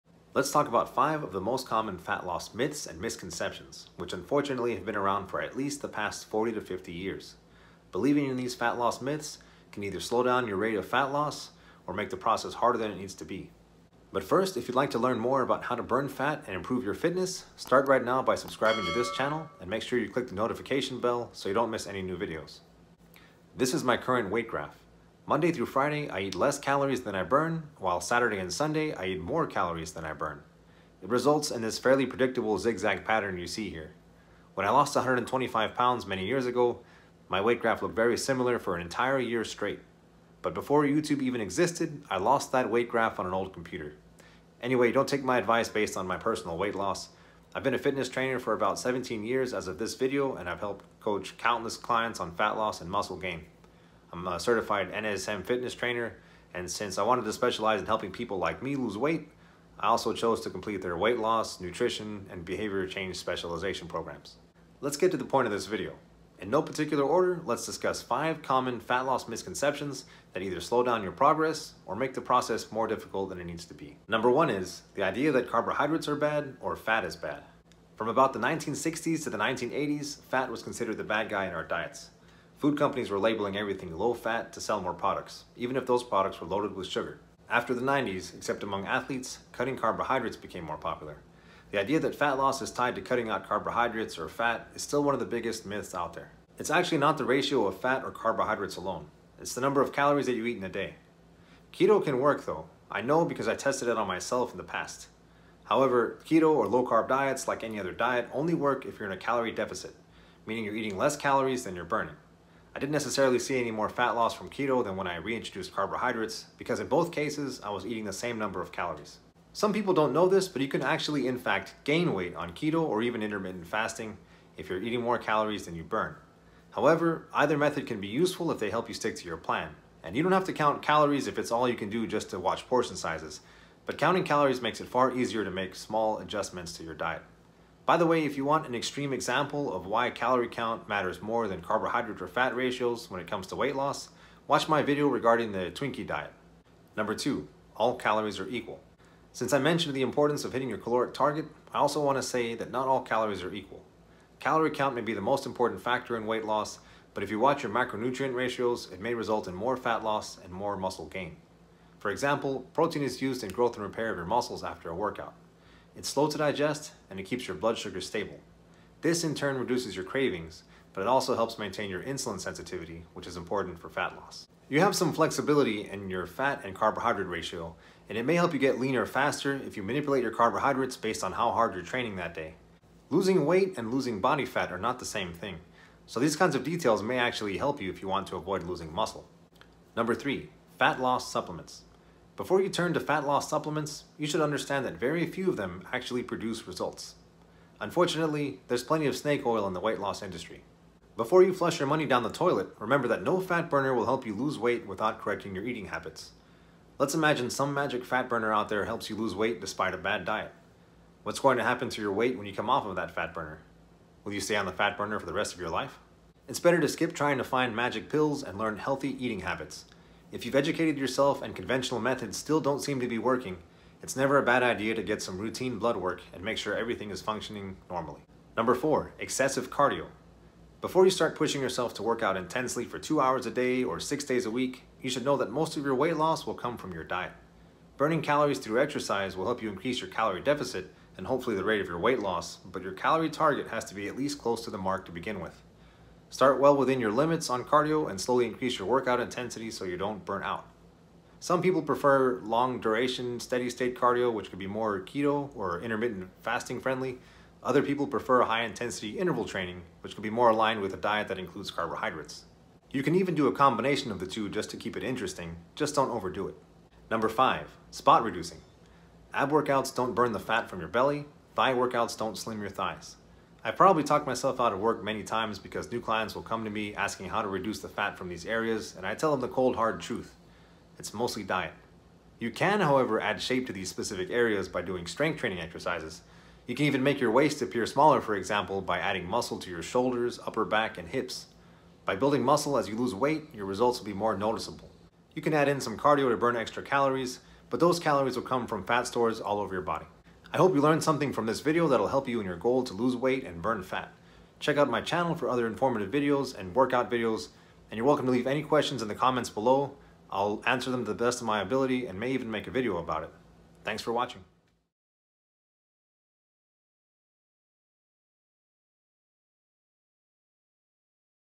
5 Common Weight Loss MYTHS
Here I explain 5 common fat loss myths and misconceptions that might slow down your fat loss progress or just make the process harder than it needs to be.

for more info about training programs.